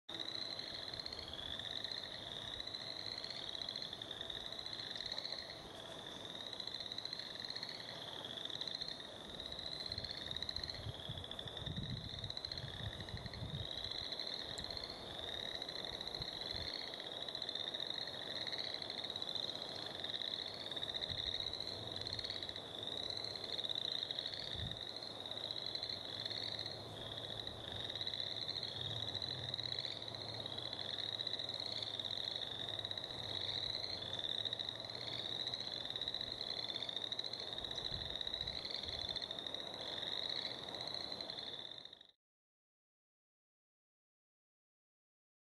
Black and White Warblers
The male Black and White warbler has a black throat.  The female  a white or light colored throat area.  Filmed spring 2014 .